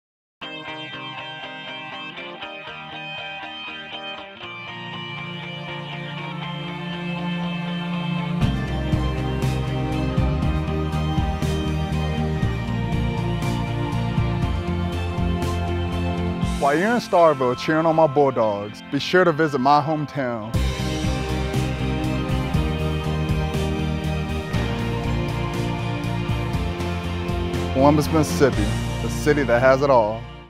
Columbus Mississippi - The City That Has It All
Former Mississippi State University (MSU) All-SEC football player Derek Sherrod takes you on a visual tour of his home town of Columbus, MS!  The City that Has It All!